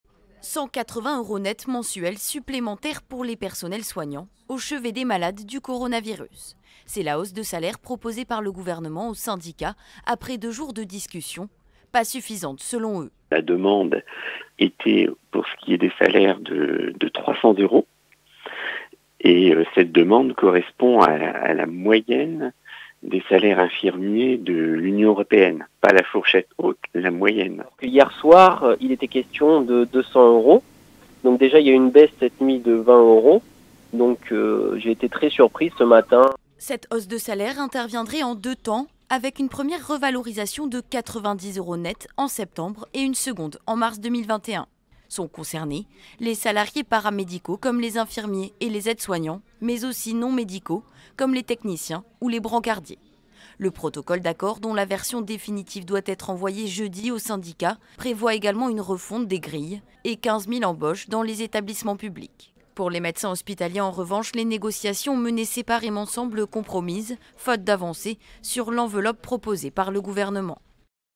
Hôpitaux : projet d'accord sur au moins 180 euros mensuels net pour les soignants hors médecins
Le gouvernement a présenté dans le cadre du «Ségur de la santé» un projet d'accord prévoyant une hausse minimum de rémunération de 180 euros mensuels net pour l'ensemble des personnels hospitaliers du secteur public, hors médecins, a-t-on appris jeudi de sources concordantes.

Cet accord, présenté aux syndicats dans la nuit de mercredi à jeudi au terme d'une ultime négociation marathon, prévoit une hausse de salaire en deux temps, avec une première revalorisation de 90 euros net en septembre et une seconde de 90 euros net en mars, a précisé l'une de ces sources.